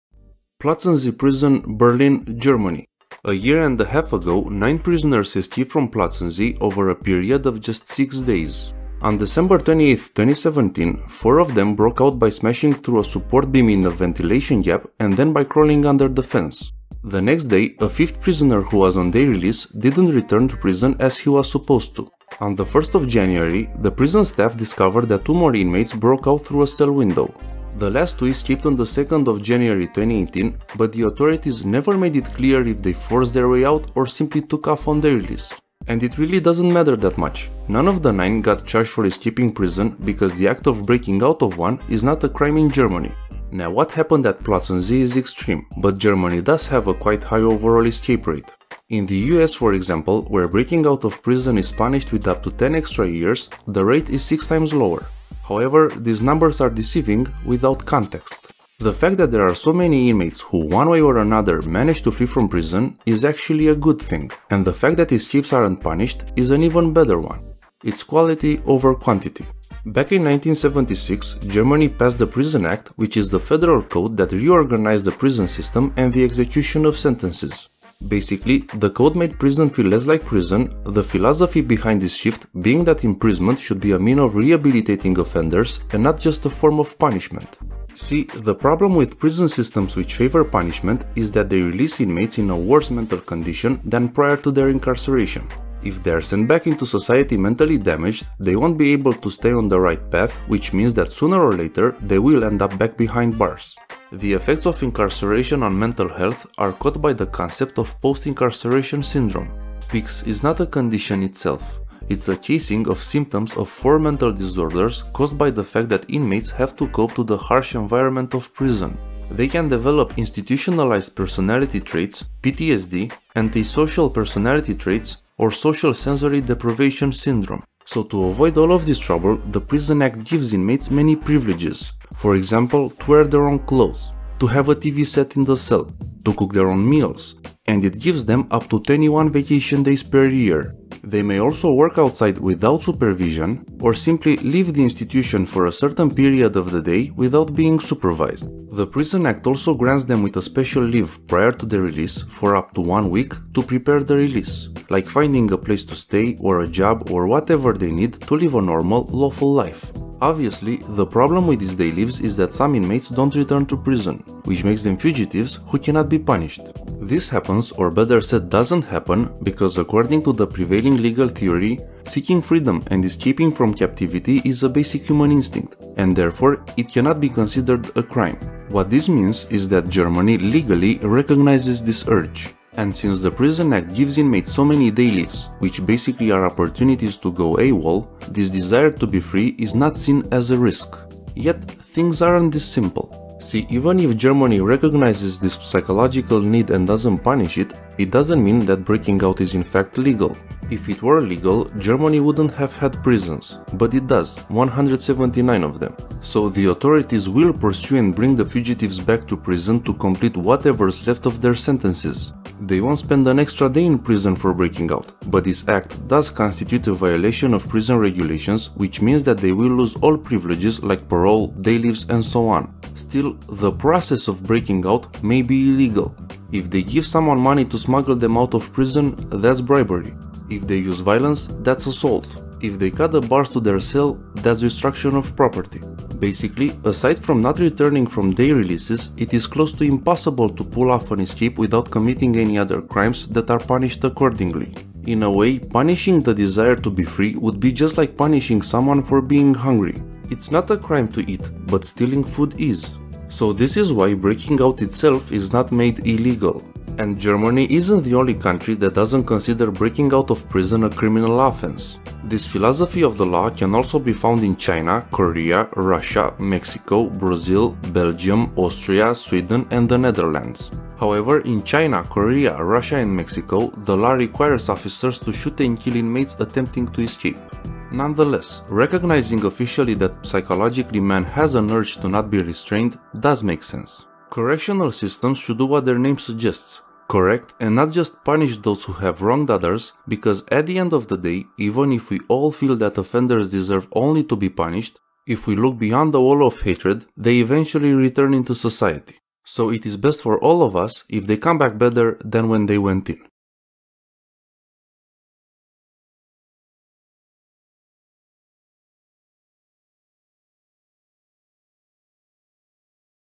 Why Breaking Out of Prison Is Not a Crime in Germany
Besides having the best high speed road network in the world, Germany also has a penal code that doesn't punish those who break out of prison.

In popular belief, sending someone to prison is a punishment for the crime that person committed. However, besides those who are sent to life imprisonment, sooner or later prisoners are released into society. The german lawmakers took this very seriously. So they built a criminal justice system that focuses more on prevention and rehabilitation, and less on retribution and punishment.

The system is designed so that it would ease as much as possible the reintegration of prisoners into society in order for them to lead a life free of crime.